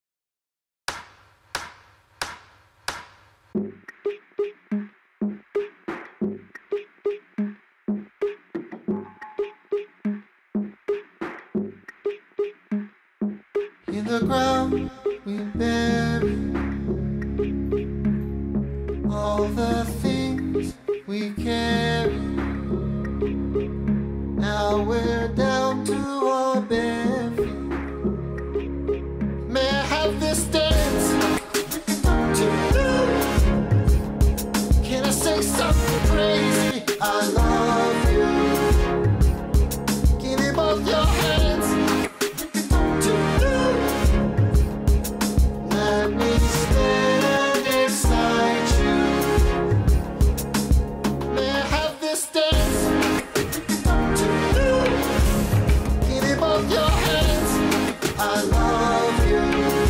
2019 Dance Recap
Quick Cut short form insight into various Los Angeles nightlife in 2019.